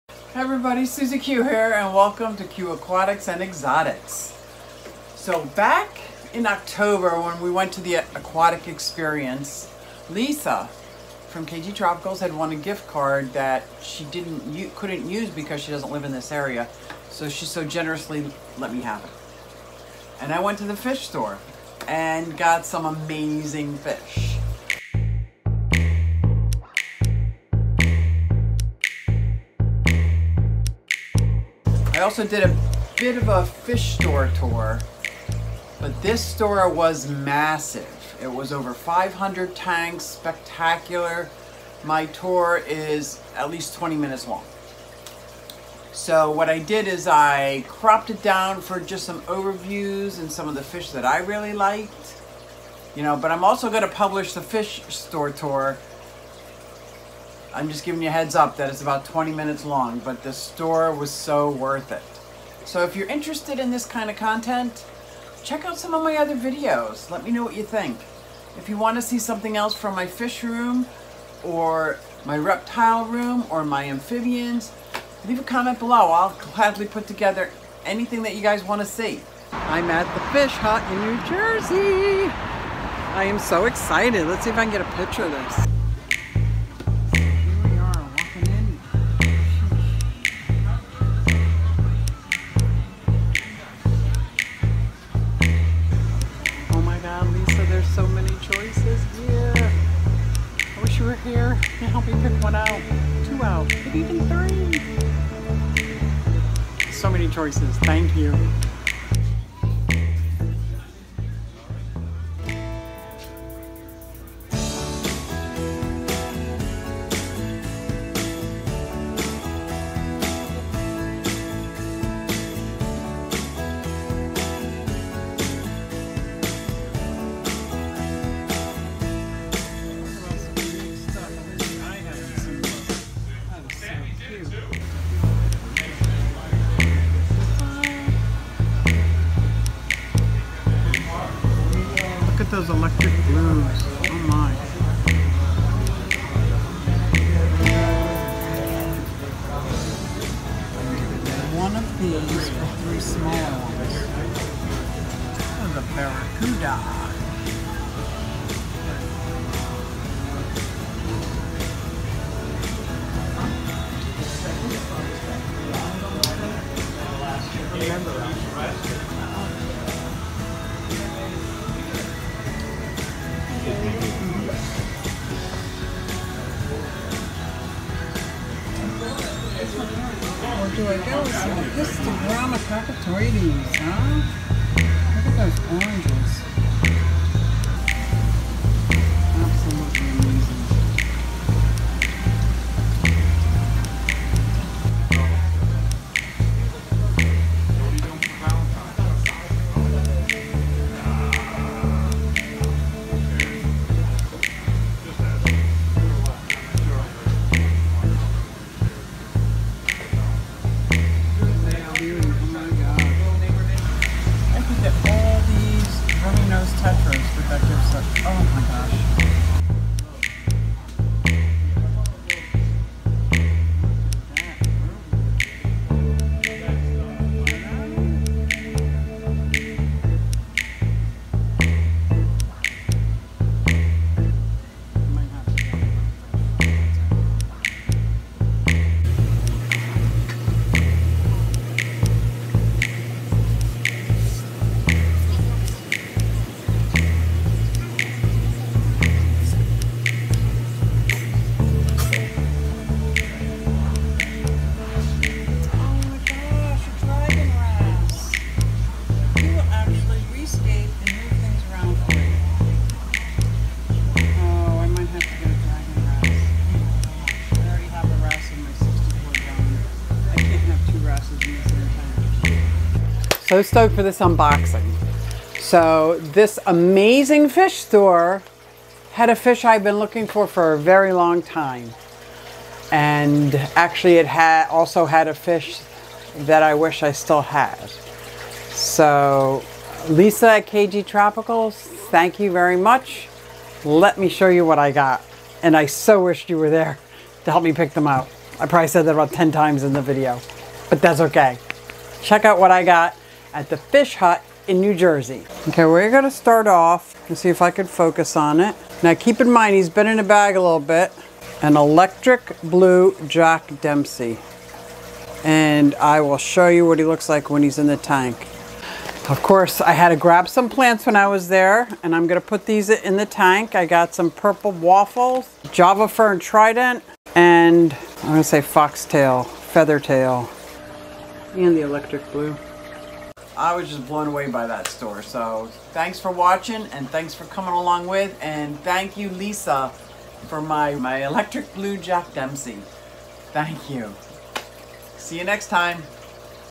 Local Fish Store In New Jersey Shopping Spree at The Fish Hut
Local Fish Store In New Jersey Shopping Spree, Come on and join me as I visit The Fish Hut in New Jersey. I have a gift certificate I have been holding on to since Aquatic Experience. I am going to get the fish of my dreams! I am getting an Electric Blue Jack Dempsey and some amazing plants! Thank you Lisa from KG Tropicals!!